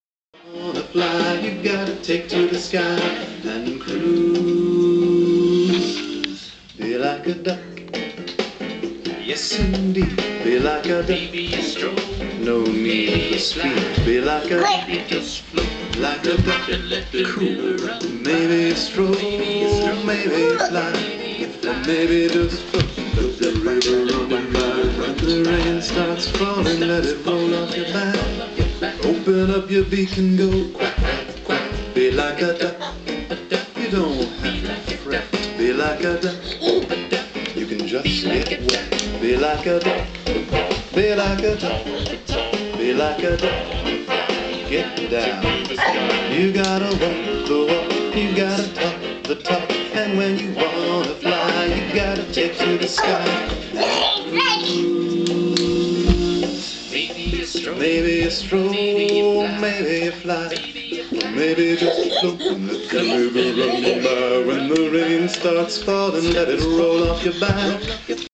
Grant's latest dance moves
Grant is getting quite animated in his dancing...he's not a baby anymore.